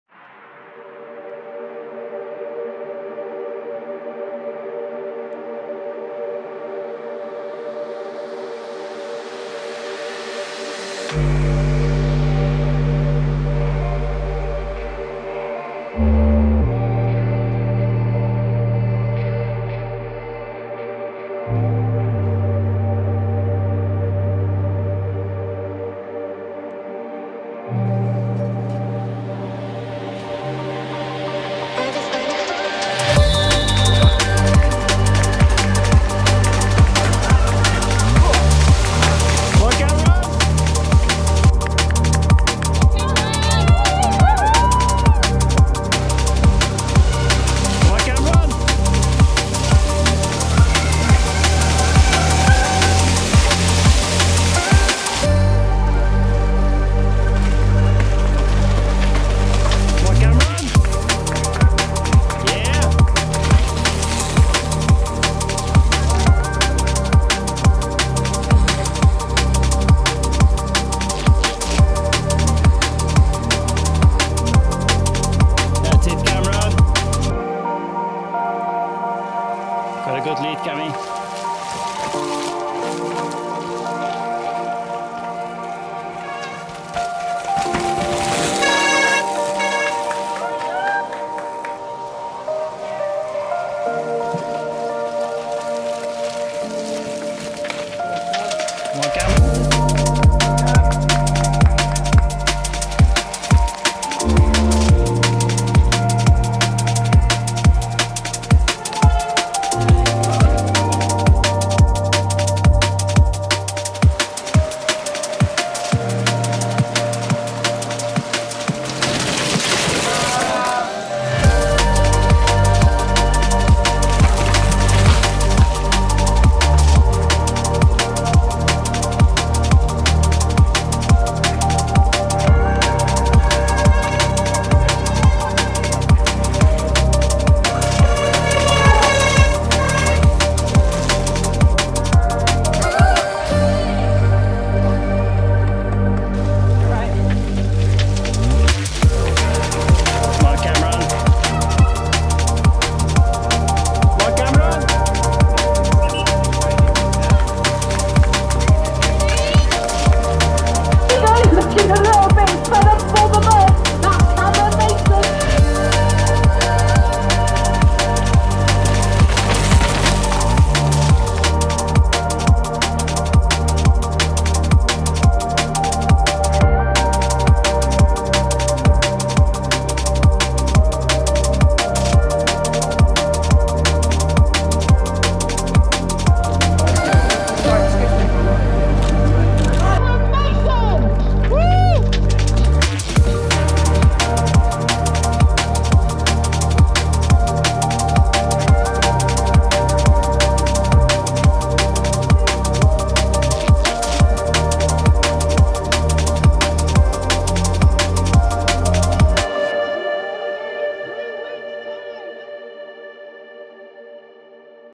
MY FIRST UCI MTB WIN!
Little highlights video from the first National MTB Round of the year at Tong, Bradford. Super muddy and techy course was good fun, especially
with the good legs I had!